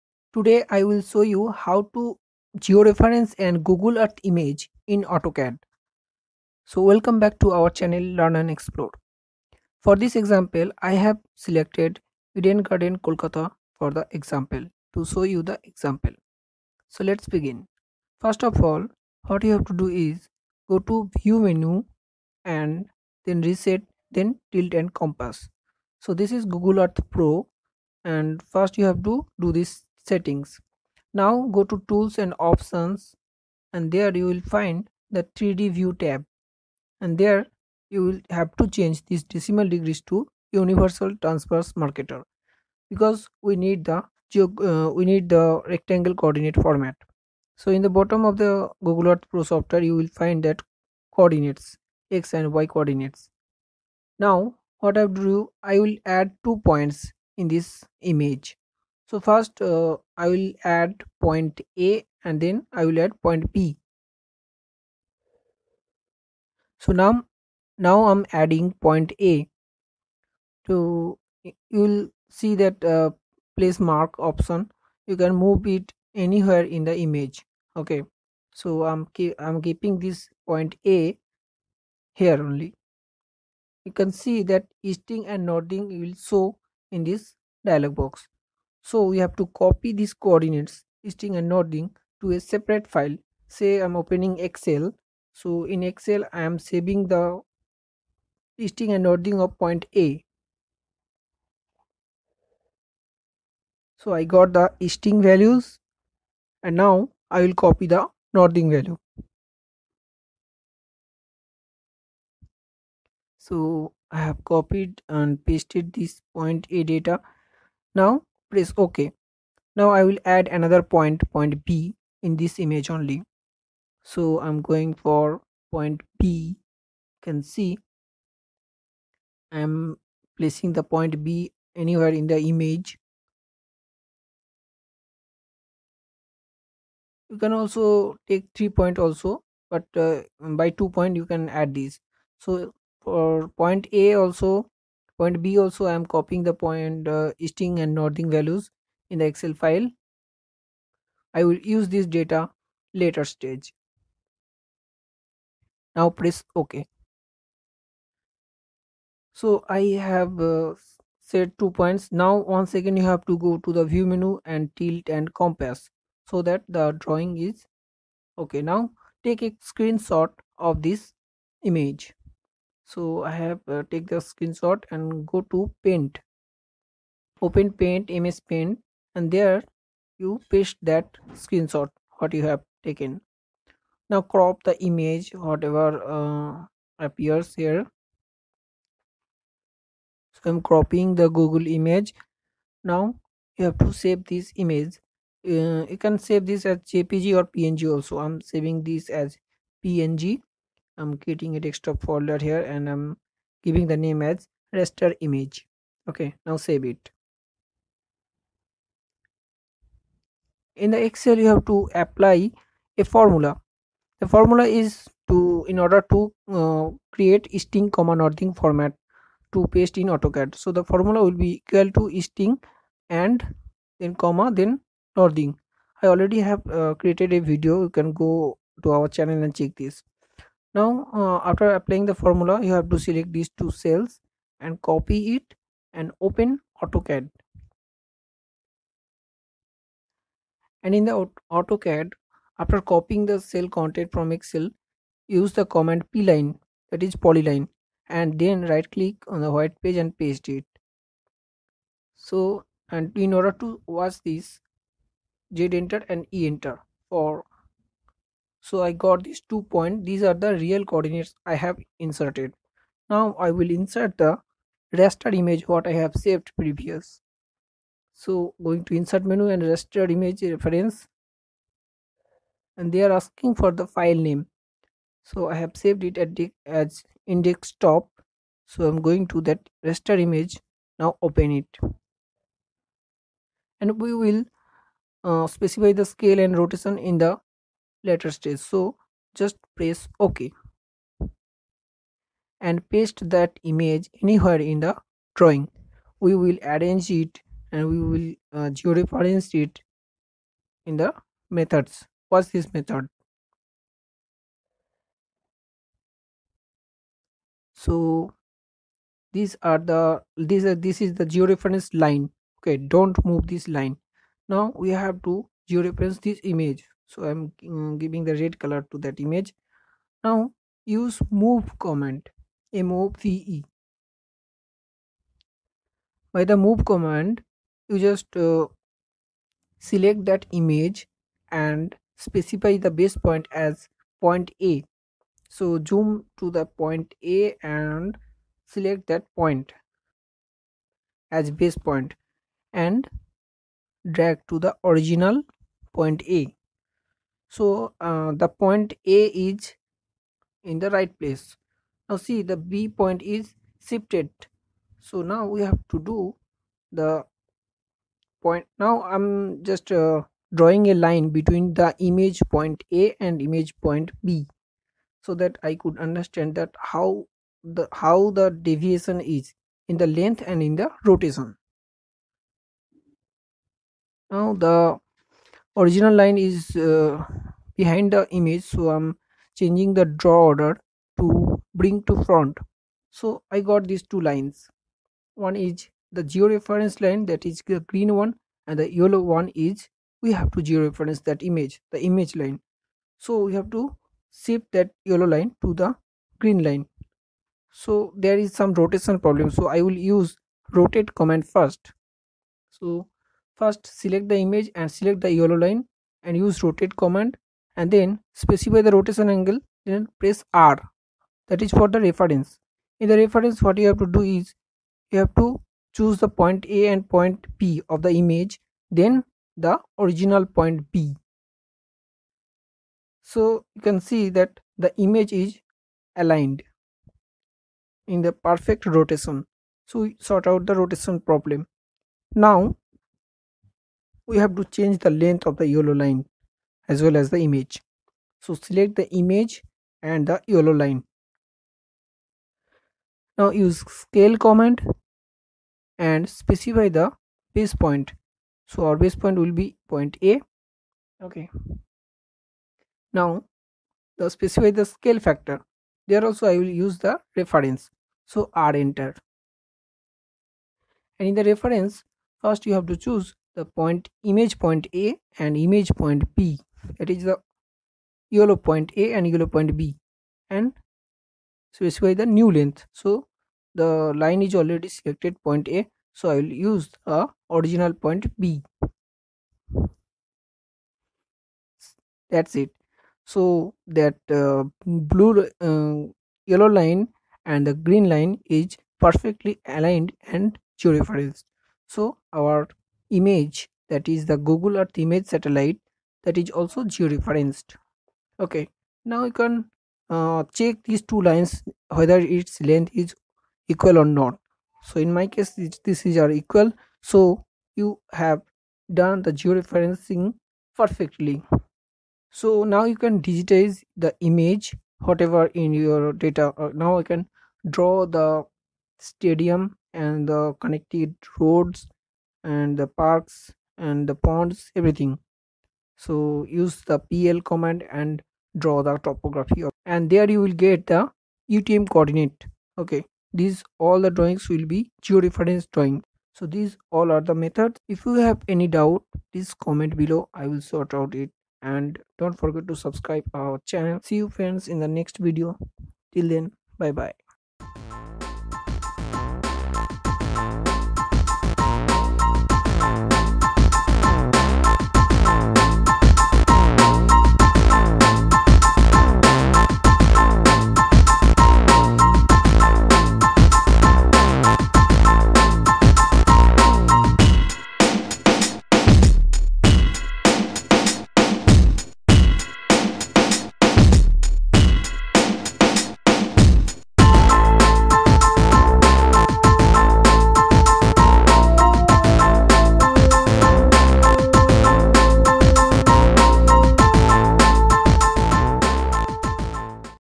Inserting Google Earth Image to AutoCAD
In this video I have shown how to overlay a google earth image in autocad.
with this method you can also georefrence an raster image in autocad.

If you want to insert an Raster image or insert a google map satellite image in autocad and then you want to make a topography map then watch this video till the end.

I have first arrange the map location in Google earth pro
then I added two placemark in the map
then i take a screenshot of the google earth image.
then i saved it by using ms paint.
then I inserted the raster image in Autocad.
then I set the rotation and scale of the raster image in Autocad
then I dizitized on the raster satellite image in order to make a topography map.

L&E Team!